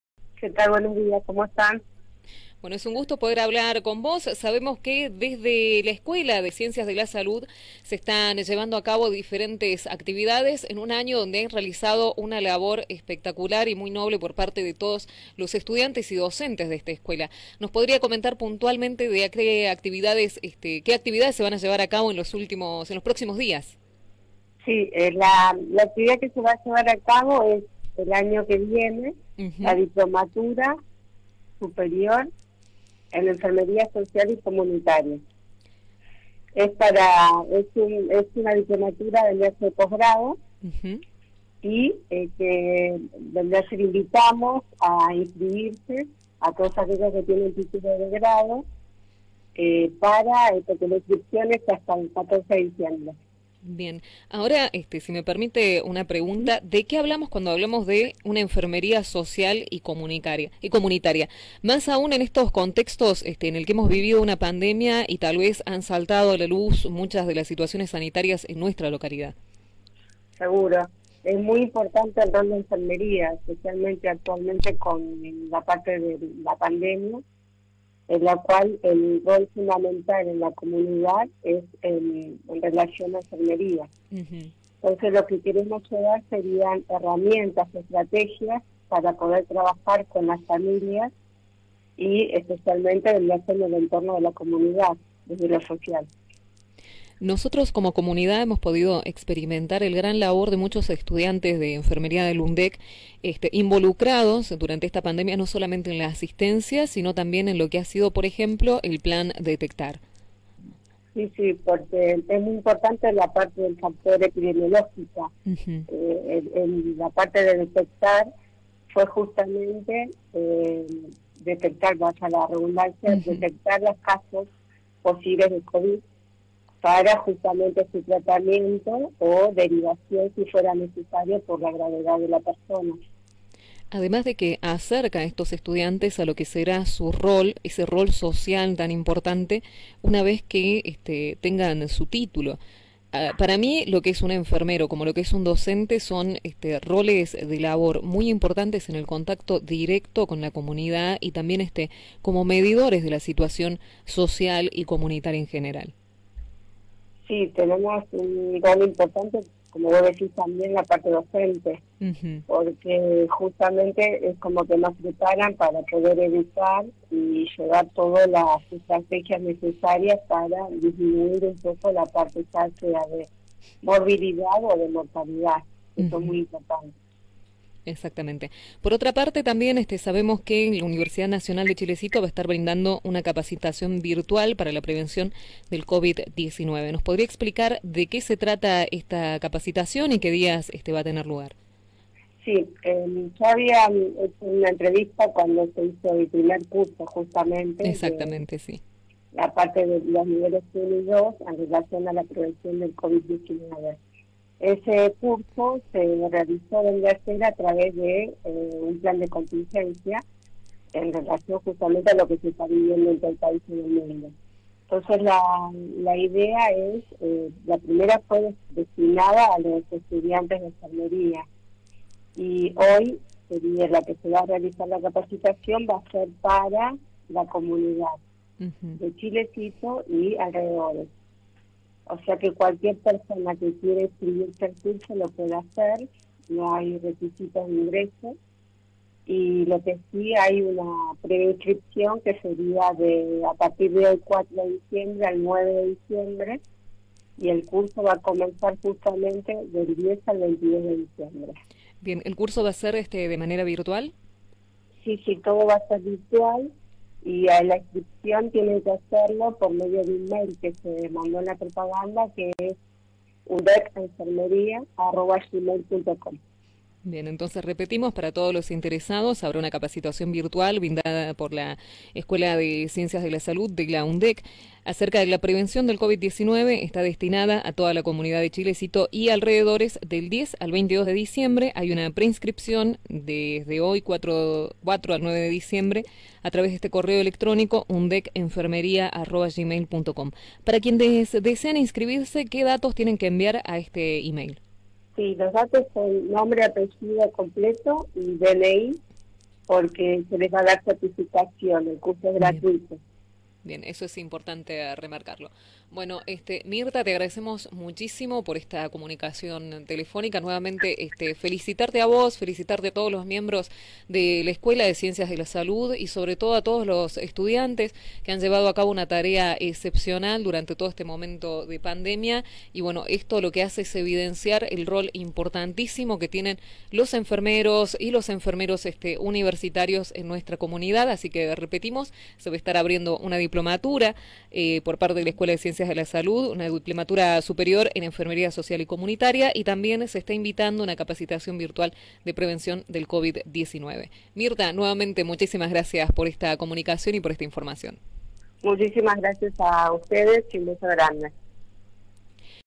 SomosLaMañana Entrevista a la Lic. Mirta Seco
Mirta Seco es Directora de la Escuela de Ciencias de la Salud de la UndeC, hablamos con ella acerca de:

La Diplomatura Superior en Enfermería Social y Comunitaria: una nueva propuesta de la Escuela de Posgrado de la UndeC dirigida a Lic. en Enfermería y a Profesionales de la Salud con título de grado.

También nos comentó sobre la Cpacitación Virtual para la Prevenciónd e COVID-19 destinada a la comunidad de Chilecito que se brindará desde el 10 al 22 de Diciembre.

📺TV: CNC grilla 906 del ViewTV
📻Radio: Fm Pucará 91.7